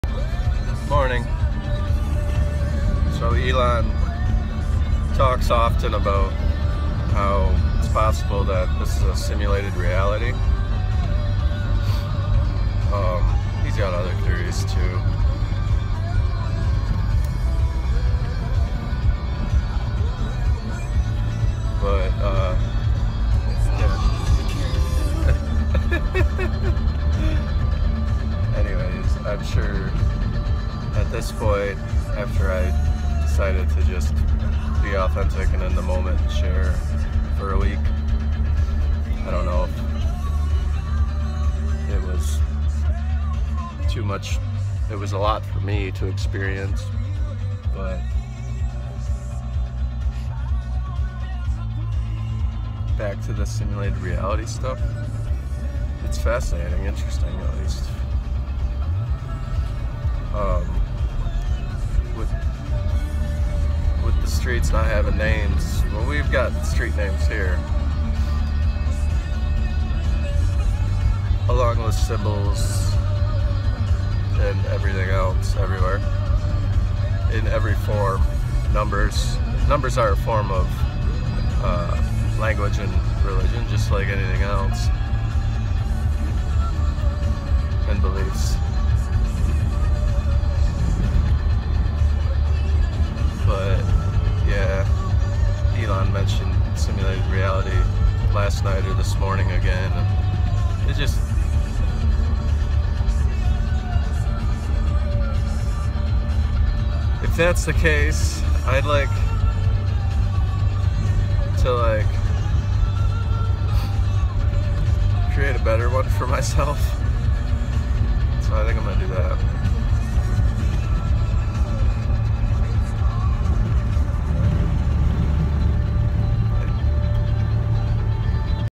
September 29, 2021
Spent a week inside of a simulated reality , program yellow and green  # streets on a morning drive, sync ro nized events - toby says hi. Not aware in the moment ect ect , two realities in this video